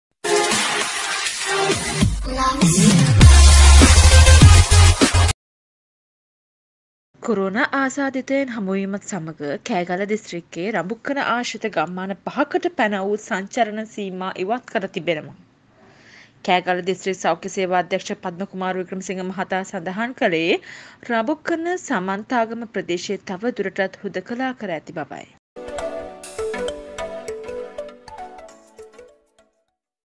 D-NEWS රඹුක්කනට පනවා තිබූ සංචරණ නීති ලිහිල් කෙරේ
රඹුක්කන ප්‍රදේශයේ ගම්මාන 5කට පනවා තිබූ සංචරණ නීති ලිහිල් කෙරේ.